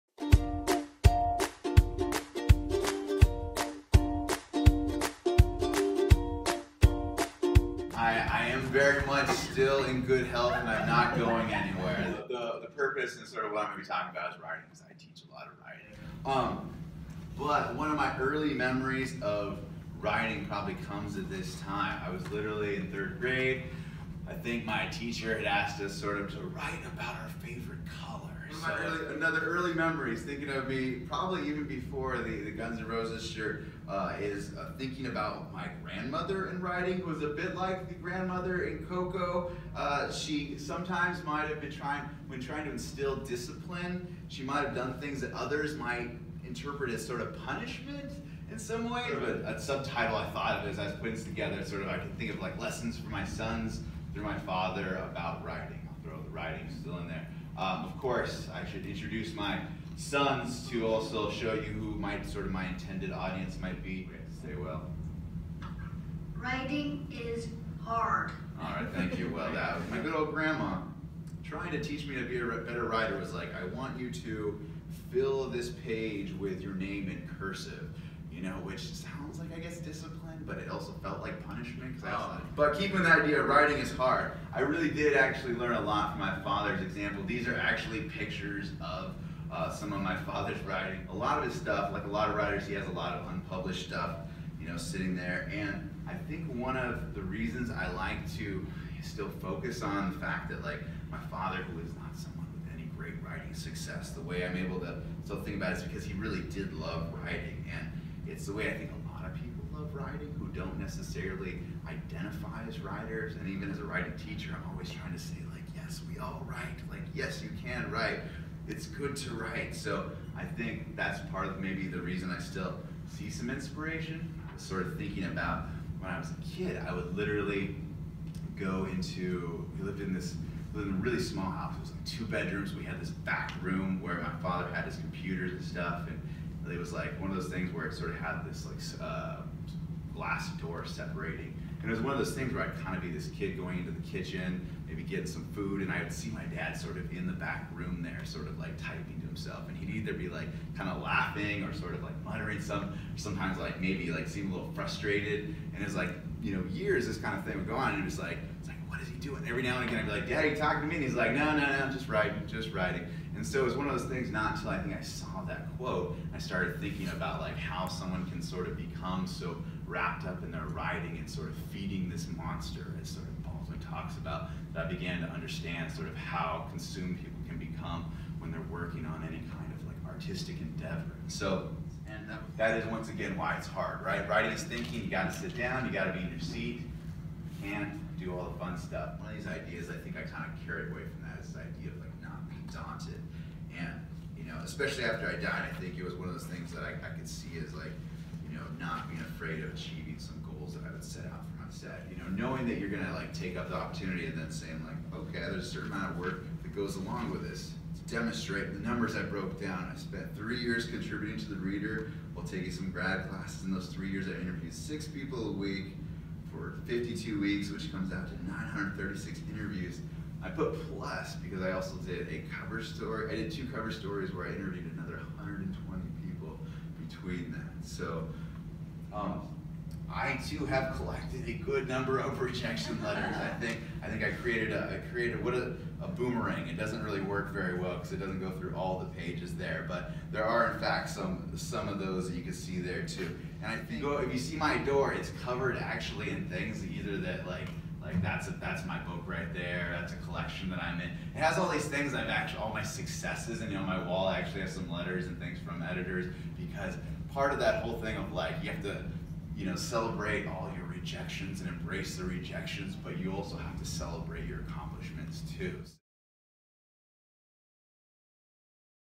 ASG's Last Lecture: Cruz Medina
A quick look at the wisdom English Professor Cruz Medina had to share about lessons he learned through writing.